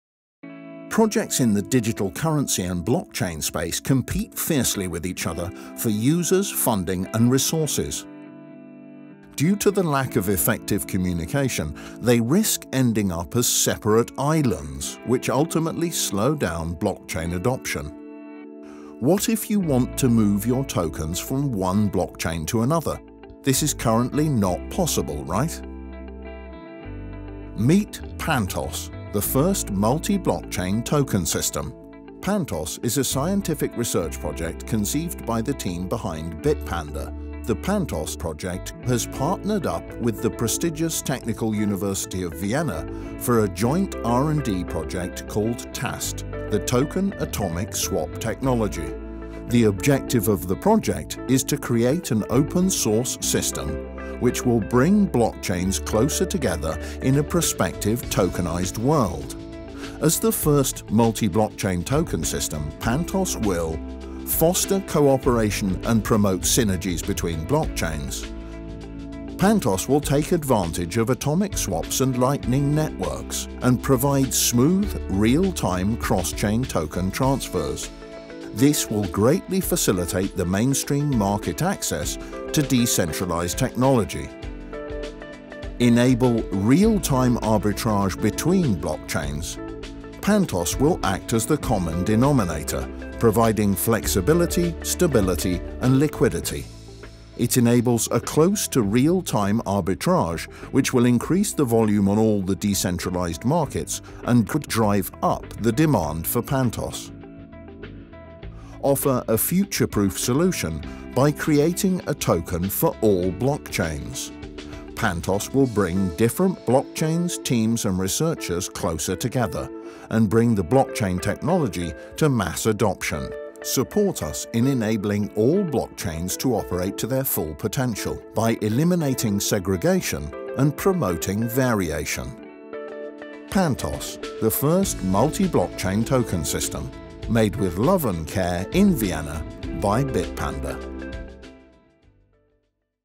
Pantos: The First Multi-blockchain Token System
Pantos is the answer to an increasingly fragmented digital currency market. Pantos is an open-source, open-innovation driven research project, which will bring all major blockchain platforms closer together, setting new standards for decentralised cross-chain token transfers.

Find out more at pantos.io

Made with ♥ and care in Vienna by Bitpanda.